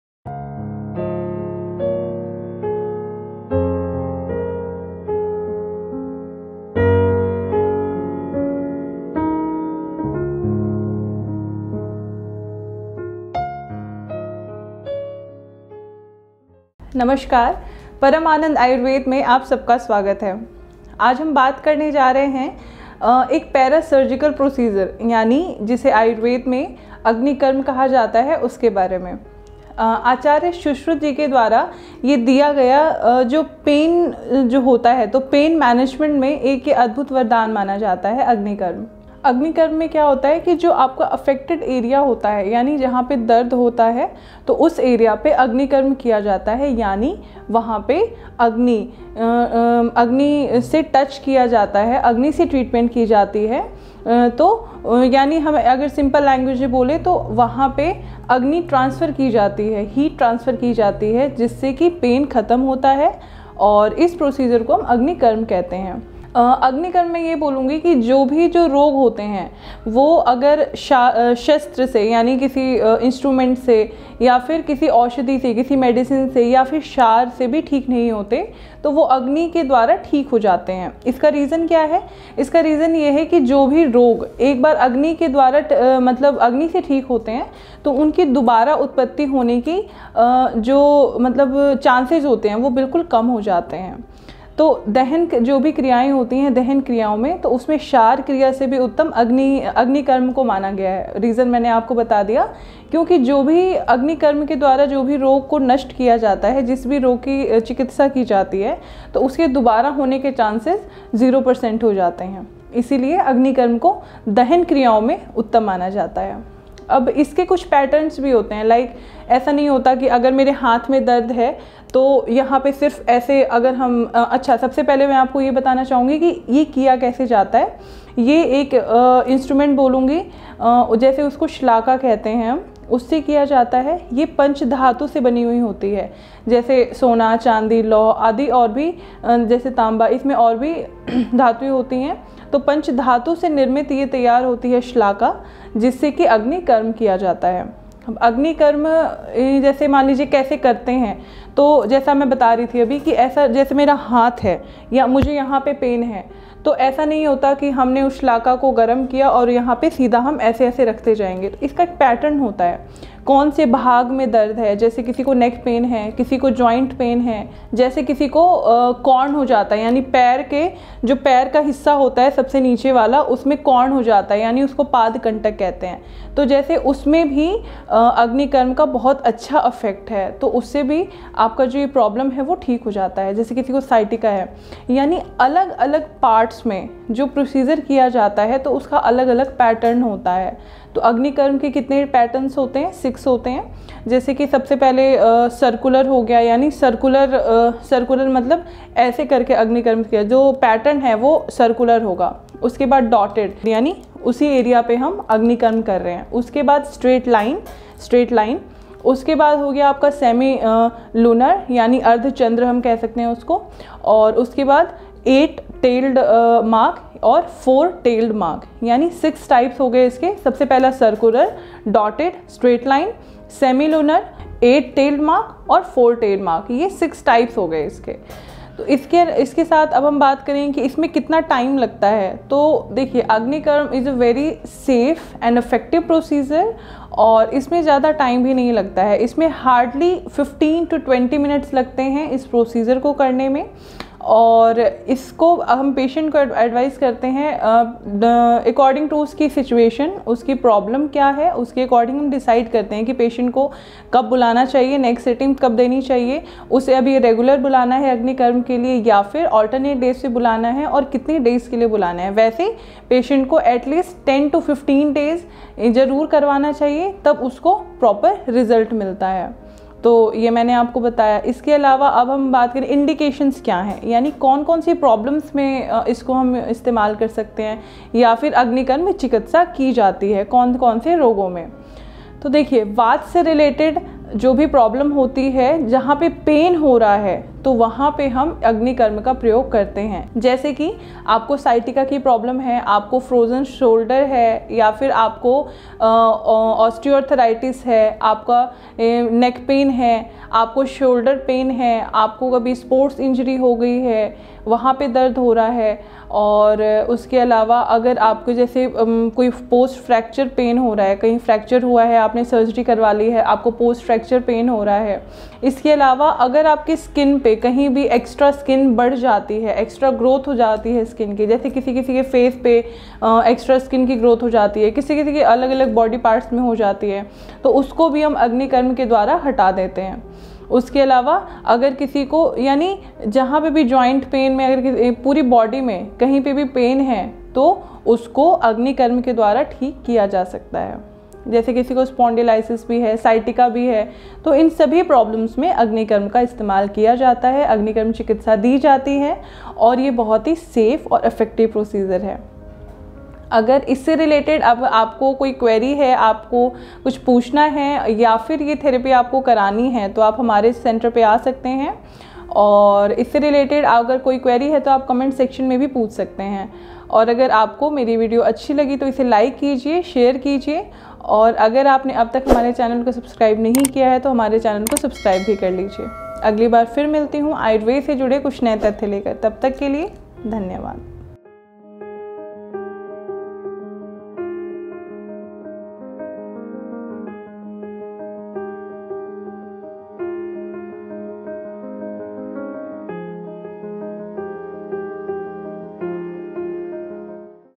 Agni Karma in Ayurveda - अग्निकर्म क्या है - अग्निकर्म की विधि और क्यों इतना लाभकारी है !
Paramanand Ayurveda Panchakarma Hospital
Address :- Plot No 2, Gopal Nagar, Near Prem Nursery Bus Stand  Main Dhansa road Najafgarh New Delhi 110043

Agni Karma is a remarkable procedure under Ayurveda. It is used in many severe and chronic problems. It is very effective in pain problems and problems related unwanted growth of skin and tissues outside body. Watch this video entirely to understand Agni Karma.

अग्नि कर्म बहुत ही अहम् प्रक्रिया है जिसमे शलखा के द्वारा हम चिकित्सा करते हैं। इससे कई जटिल बिमारियों का इलाज होता है। इसमें ज्यादा समय और खर्चा नहीं लगता है।